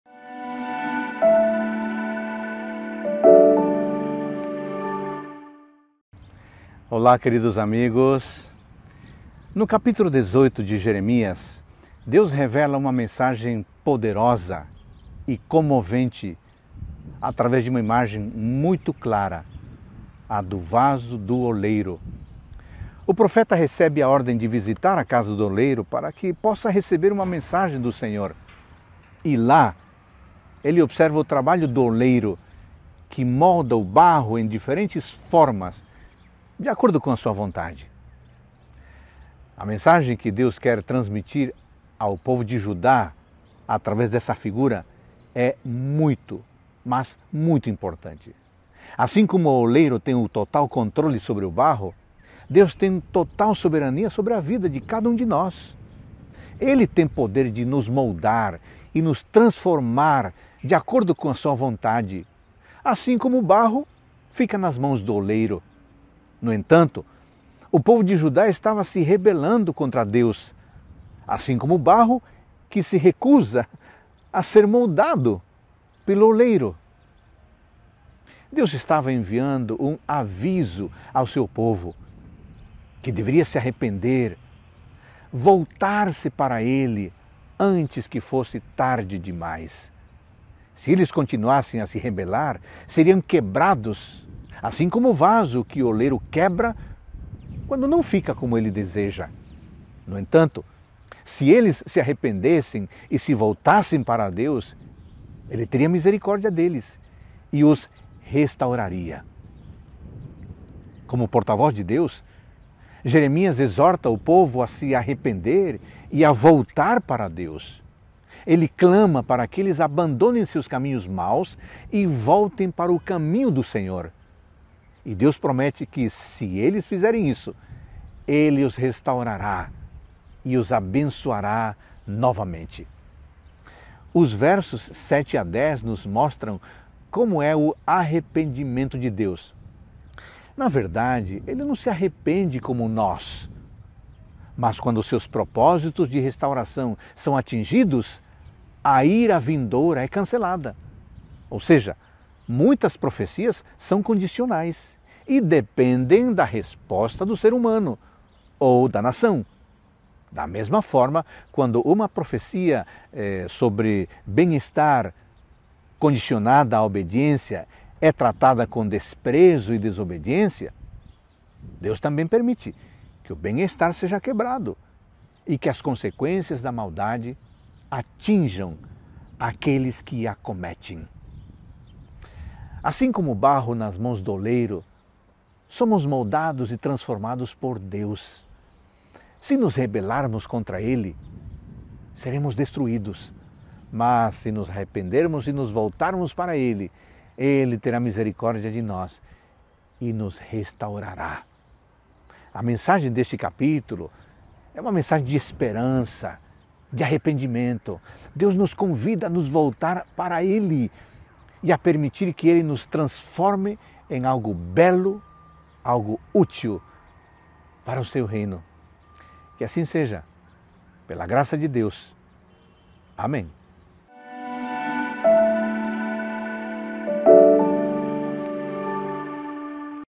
Jeremias 18 - Reavivados por sua Palavra
Assim como o barro nas mãos do oleiro, somos moldados e transformados por Deus.